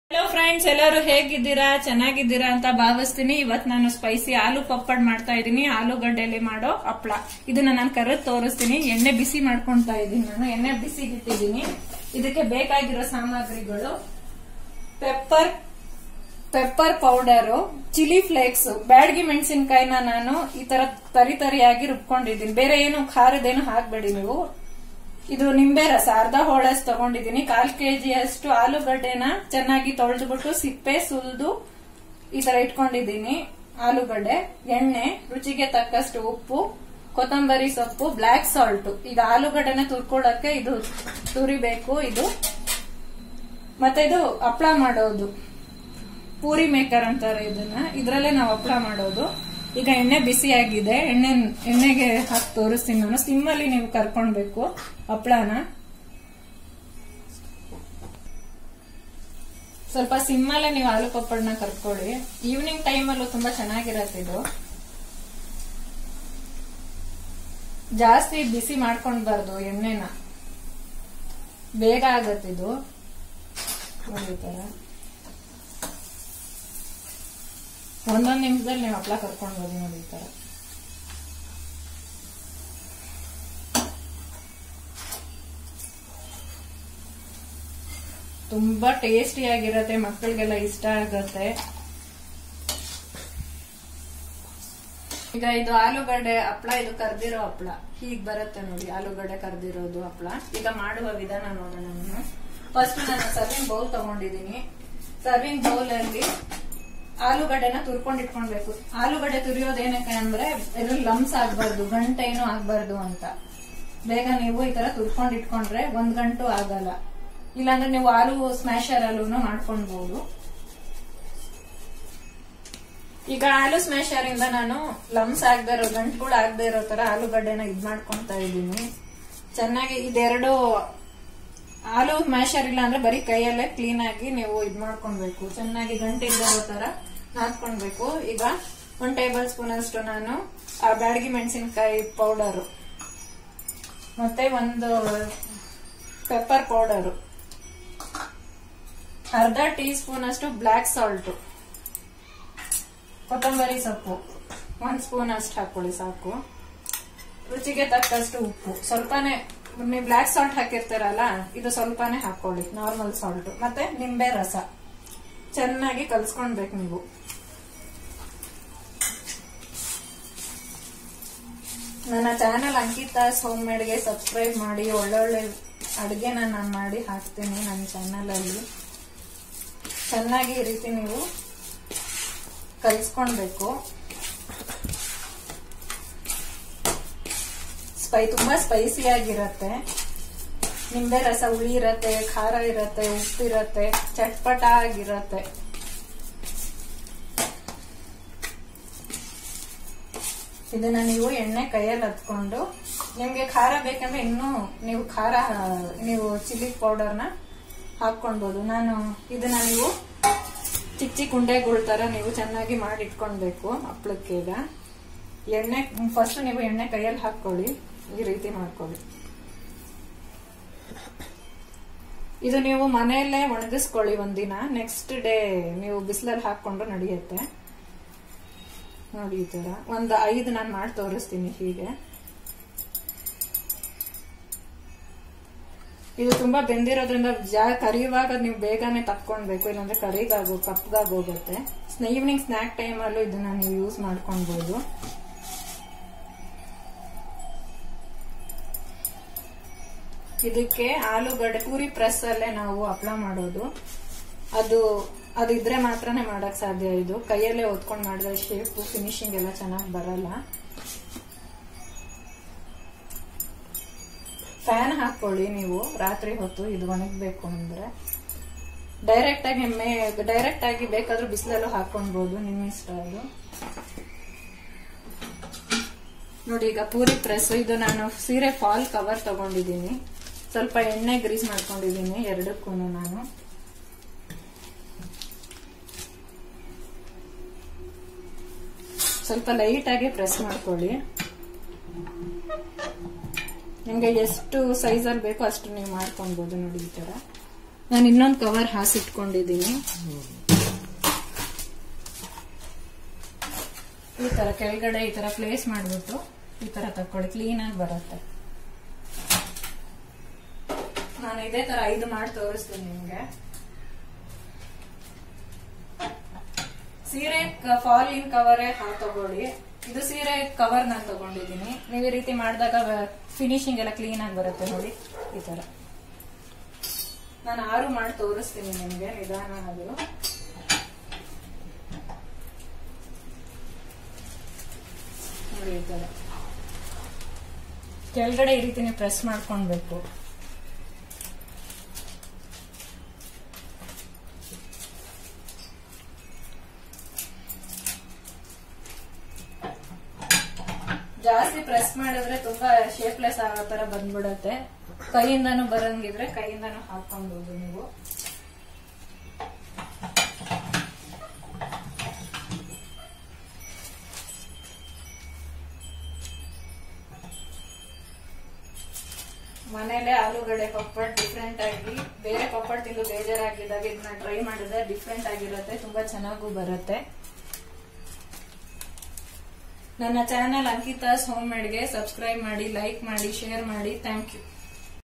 Spicy Aloo 🌕🌖Papad, Potato Papad How To Make
Every One Favourite Aloo 🌕 Papad And Potato Papad at Home In Ankithas Home Made 🤩
Ingredients
Boiled Potatoes 🥔🥔🥔
Chilli Flakes 🤗🤗🤗
Black Salt ☺️😊🥰😍
Normal Salt ☺️☺️😊🥰😍🤩
Lemon 🙄🤩🤡😝🥵😰
Cooking Oil 😝🥵😰🥵😰
Pepper Powder 🧐🥺🤔😰
Coriander leaves 🤗🤗🤗🤗🤗